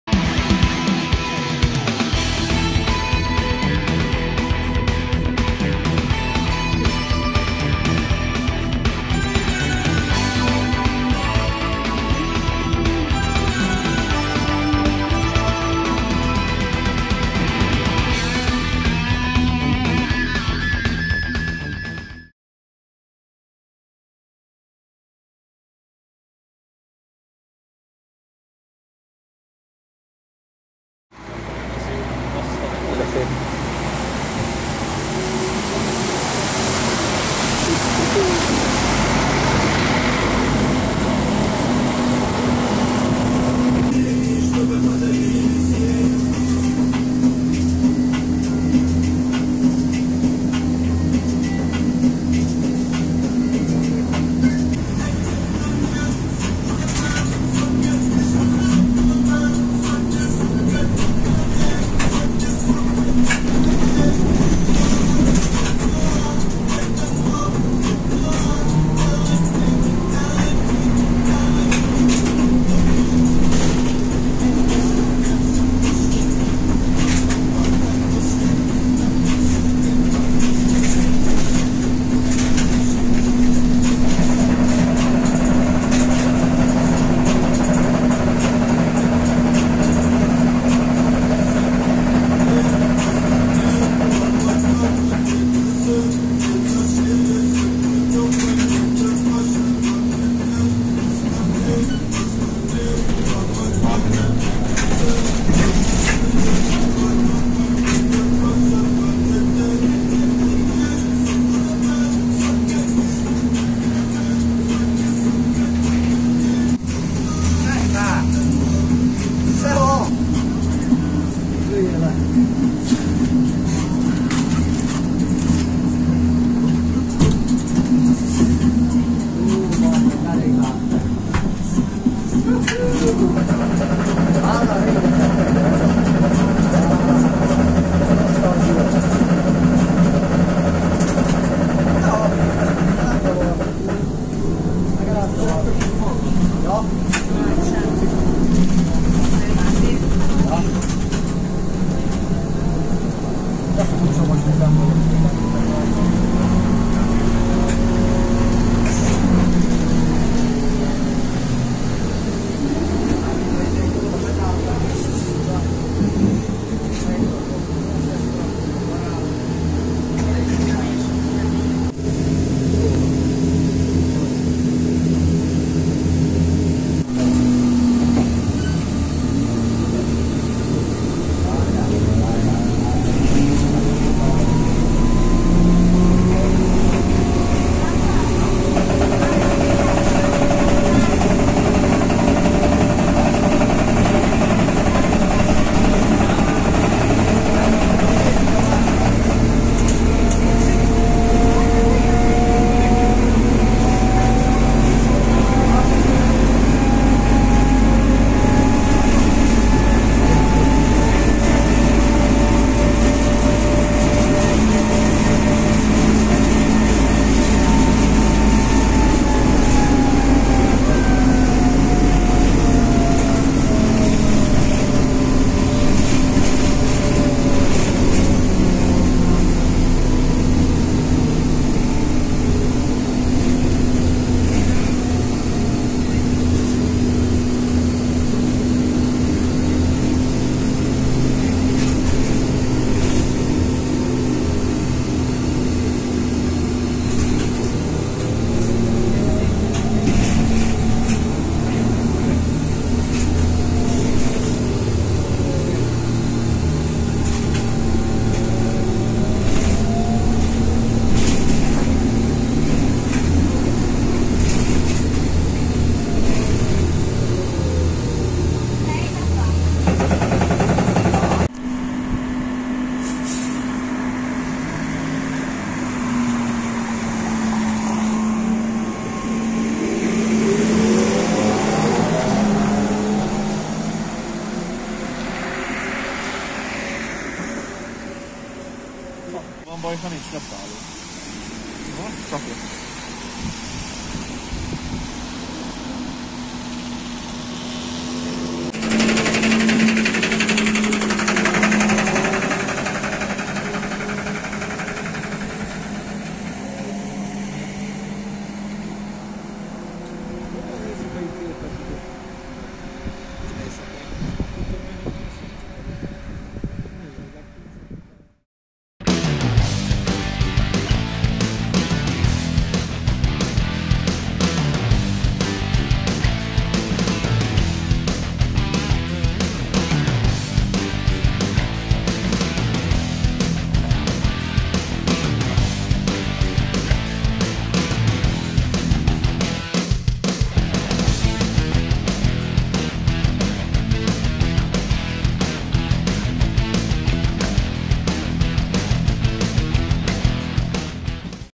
[Full HD] SzKT ZIU9 - 9-153 fénytroli (5)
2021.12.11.
Bartók tér - Torontál tér

A videó aktualitásával "kissé" megkésve de törve nem, íme Szeged egyik nosztalgia trolibusza amit régóta akartam már fénytroliként elkapni. Hangulatos kívül-belül, jó volt utazni vele még arra a kevés időre is.